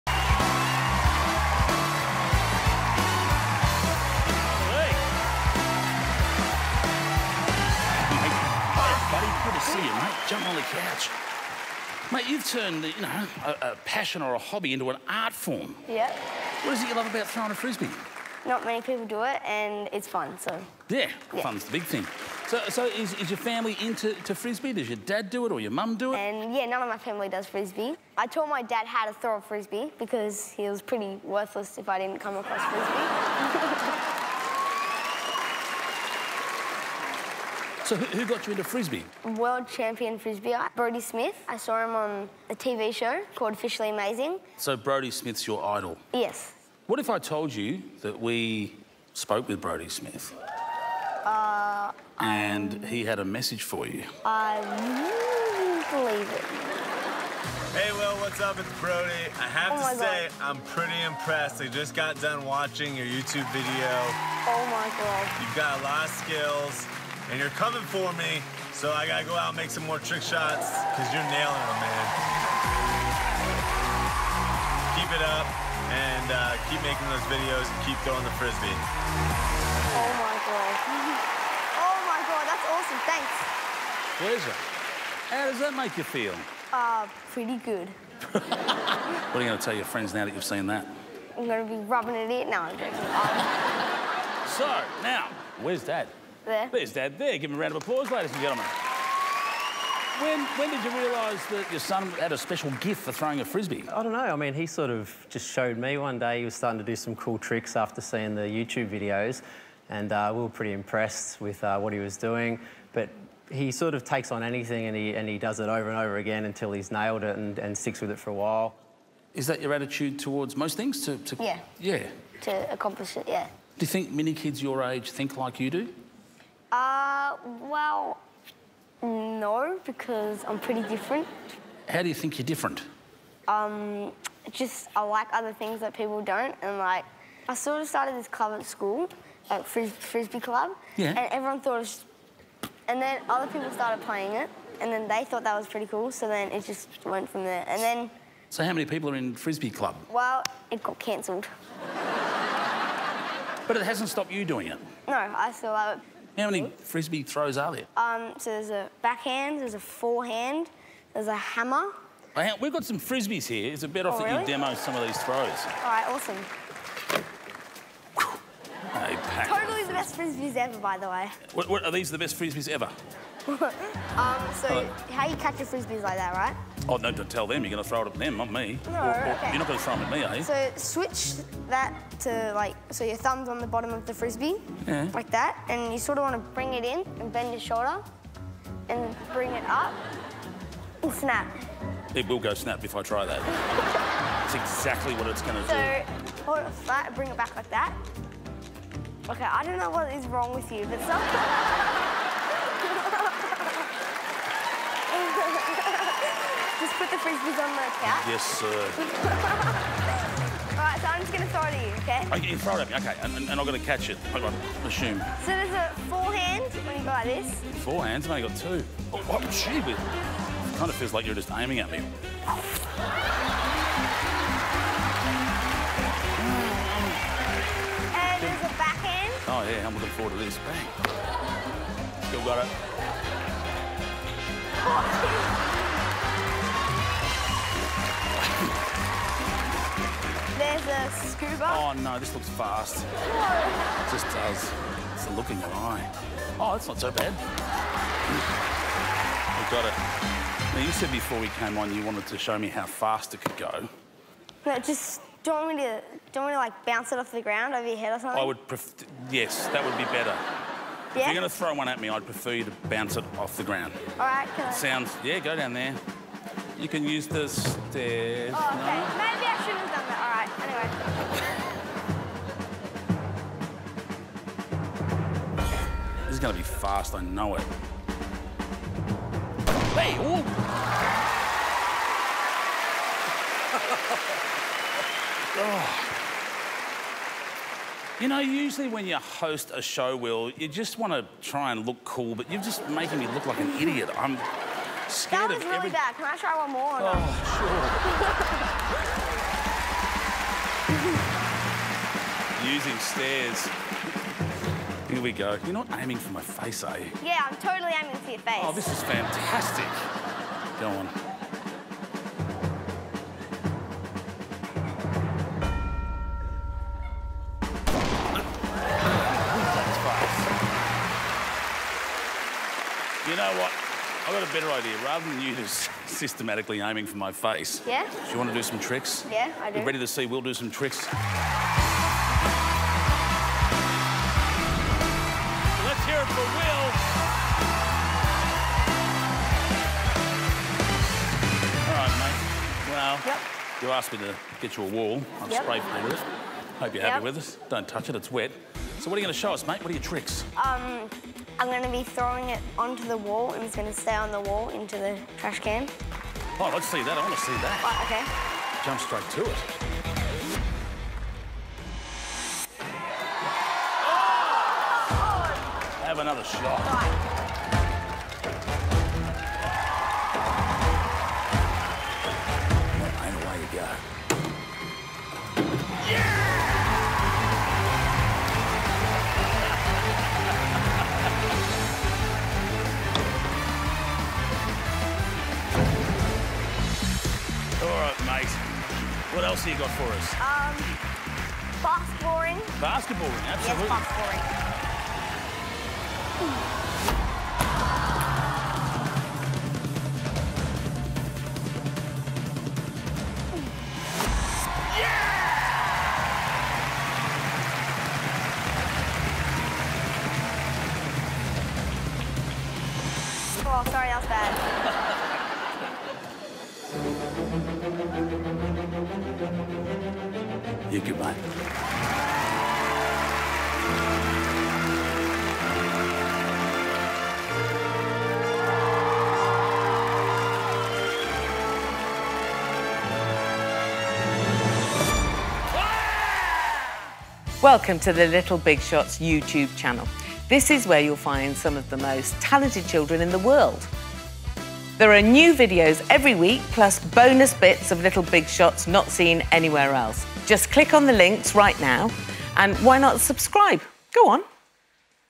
Frisbee Tricks Tutorial With Will
Meet Will, the ultimate 9-year-old frisbee trickster as he discusses how he got into throwing frisbees and teaches Shane a few trick shots.